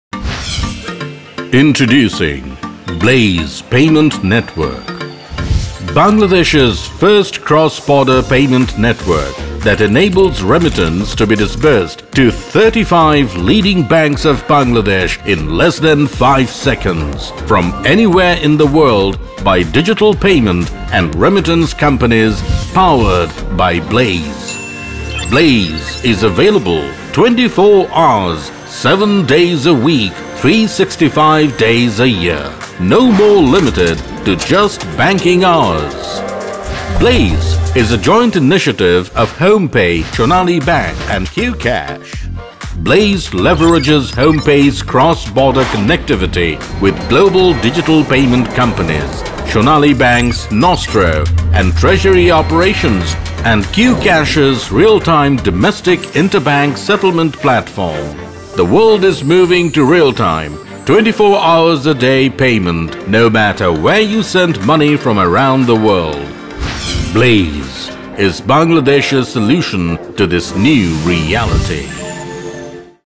Blaze International Payment and money transfer app
Blaze is Bangladesh’s first instant, 24×7, 365 days cross-border payment network
FAQ
Is Blaze a remittance app? Can I download it anywhere?
-- No, Blaze is not a remittance app. Blaze is a service that connects global remittance companies with 35+ banks in Bangladesh. Once connected, money can be sent to Bangladesh within 5 seconds. Blaze runs 24 hours a day, 7 days a week ensuring seamless financial service at your fingertips.

I'm Probashi/expatriate, how can I use Blaze service now?
-- We are extending our partnership with global remittance companies/services.

What if my remittance company is not Blaze enabled?
-- You may use the apps above or also you can request your remittance company to contact Blaze to use Blaze service.

What remittance companies are going to be supported by Blaze?
-- We are constantly extending our partnership with the leading remittance platforms. The list is growing as Blaze is integrating with many popular payment services.Please keep an eye on our website and social media for live updates.

I'm a local Bangladeshi bank, I want to connect to Blaze. What should I do?
-- Blaze is very easy to integrate. Our technical team will provide you necessary supports for the integration process.